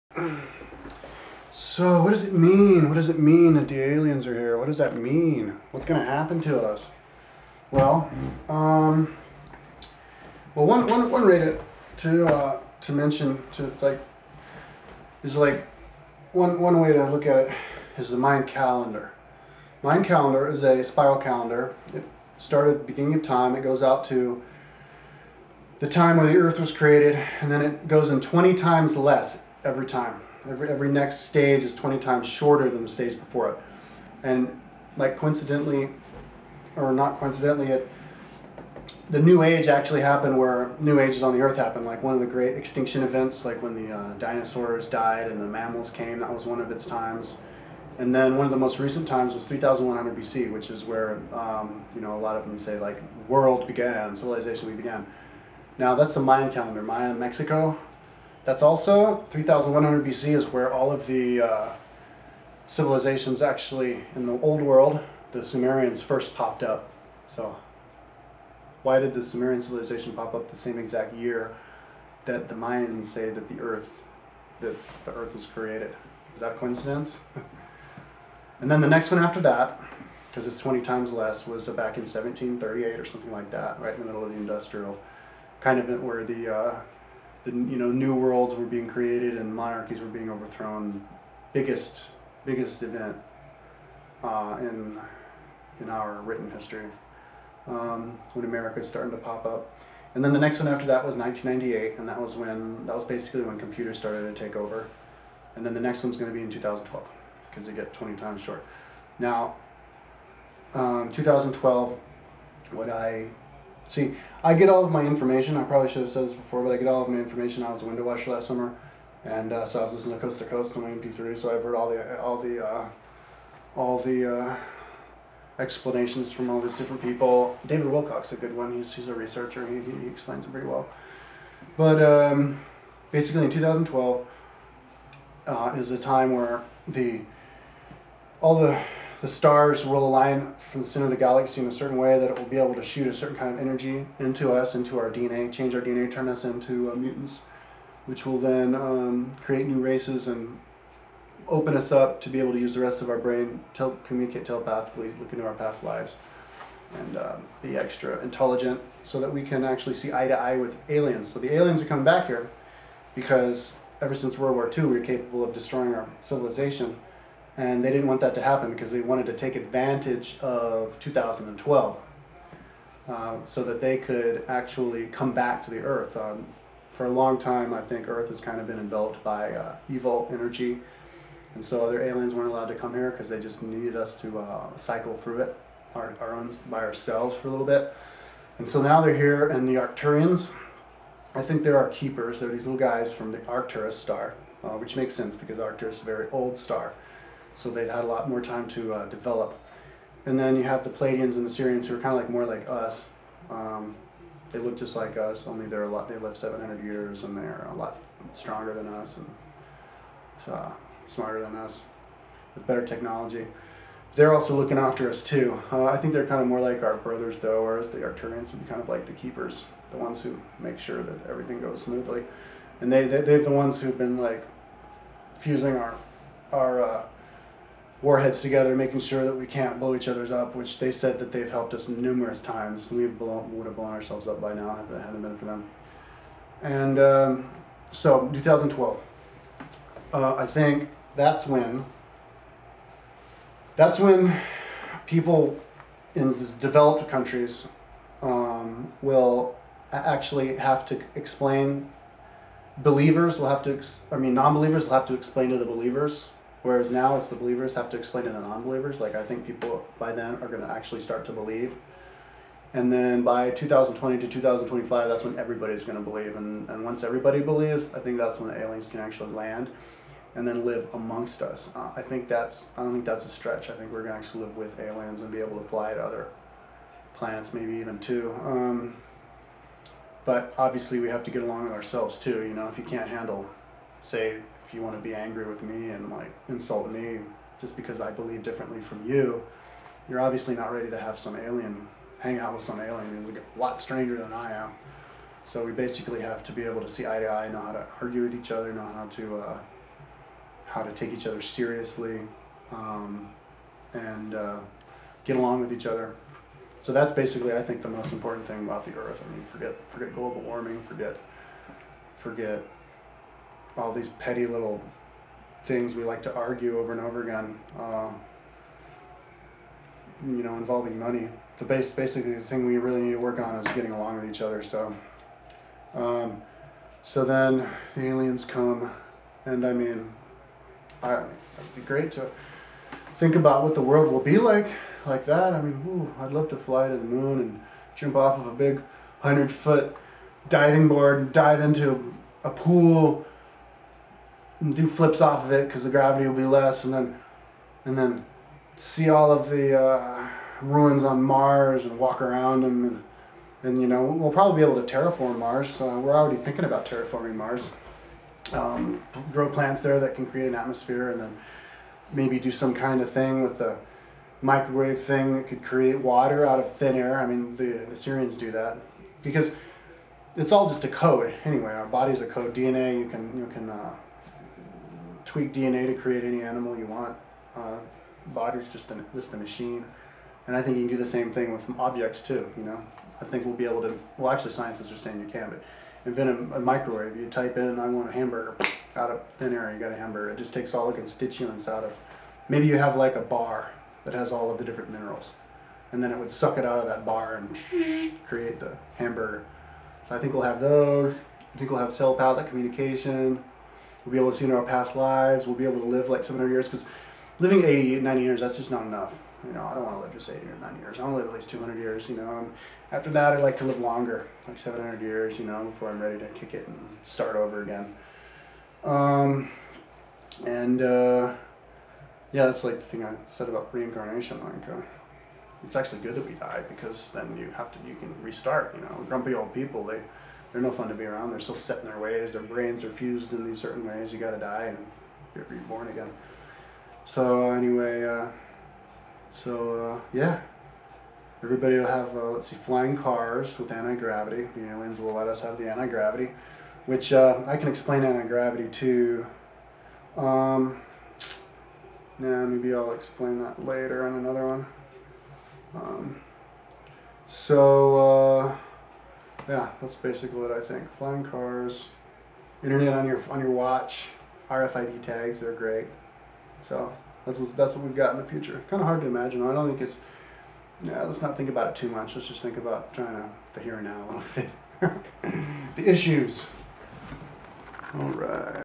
What does the fact that the aliens are here mean?
Rambling on about what it means that the aliens are here and contemplating the future with them.